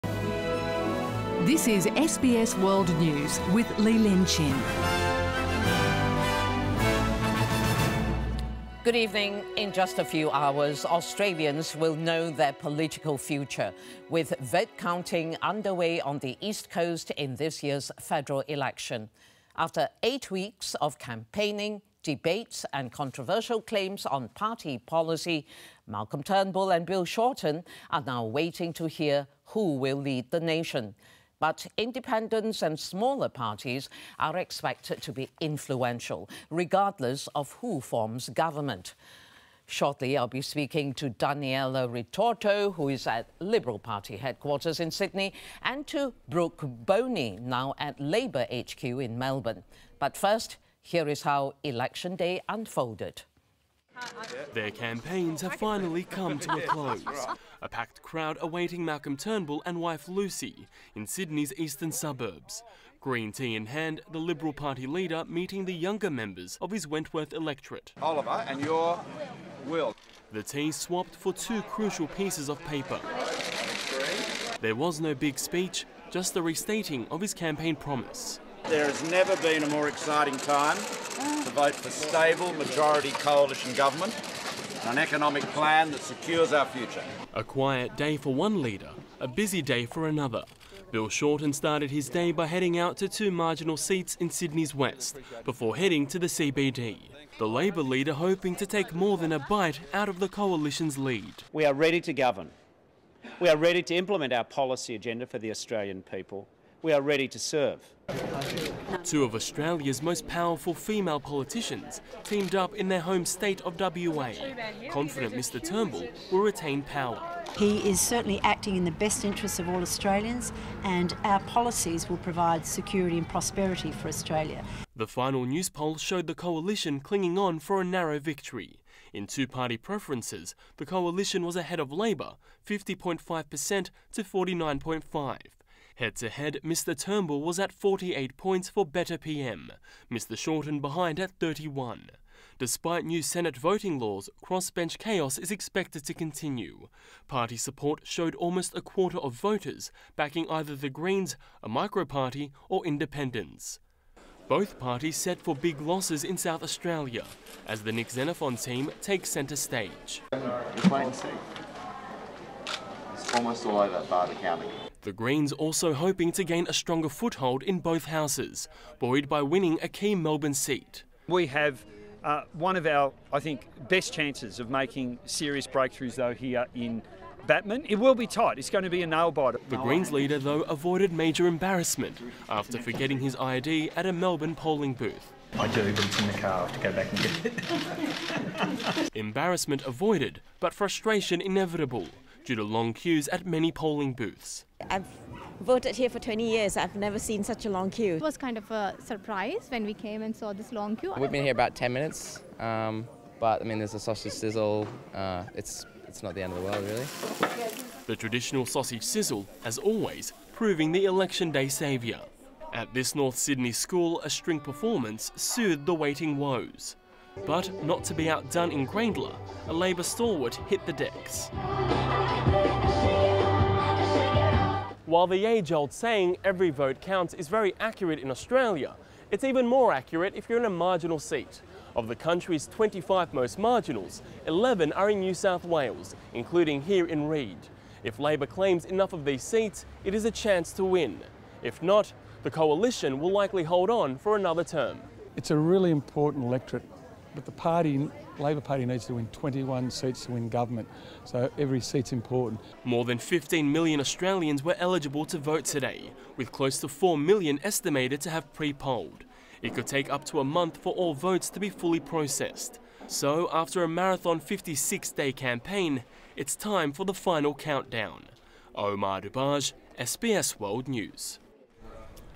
Australians head to the polls for 2016 election
In a matter of hours Australians will know which party will lead them for the next four years.

Omar Dabbagh has visited key marginal seats across Sydney, and filed this report.